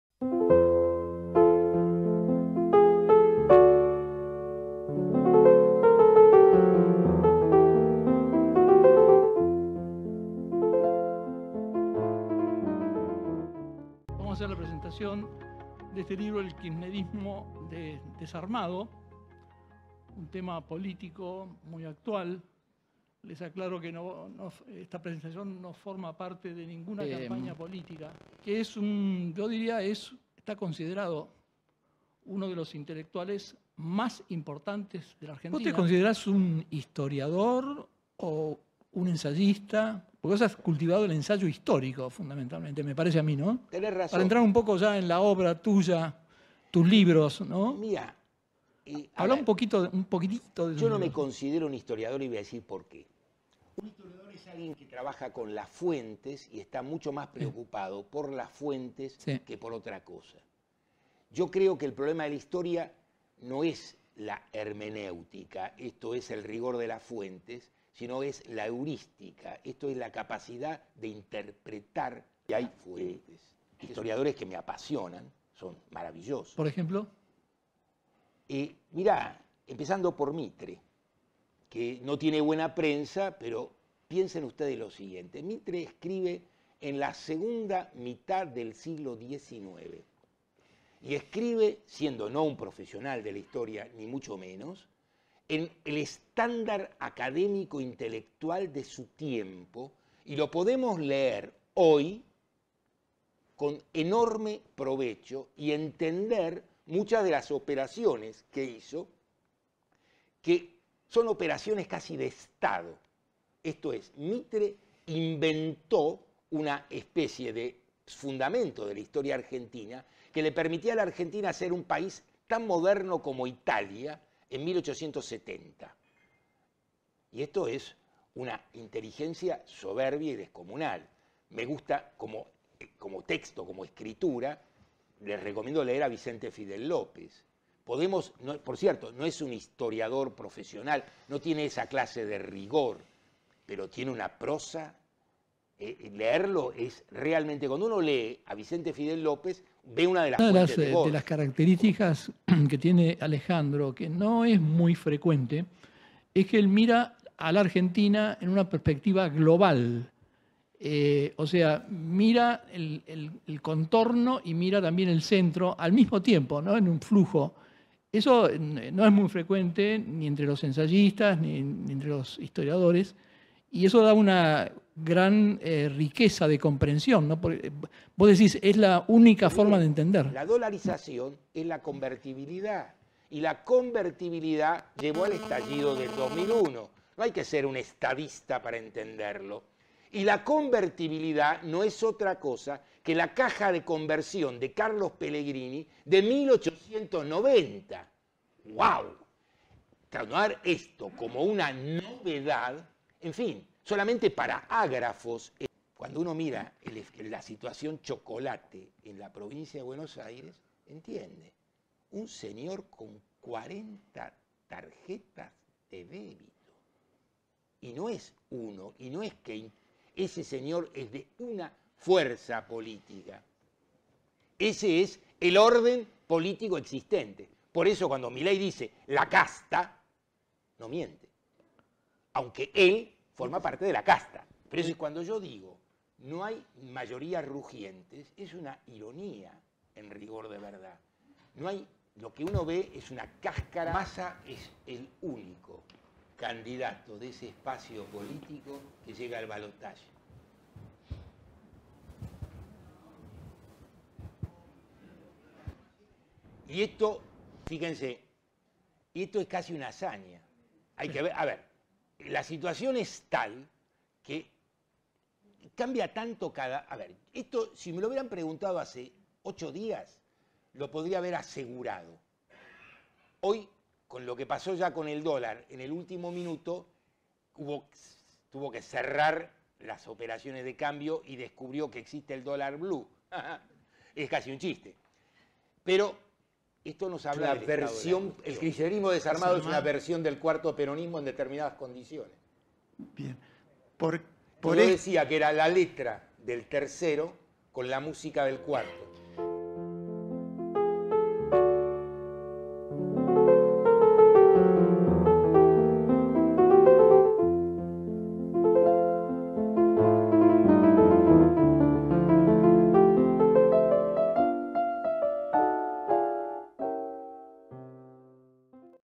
Alejandro Horowicz. Entrevista y presentación del libro "El kirchnerismo desarmado"
El Dr. Roberto Bosca entrevista a Alejandro Horowicz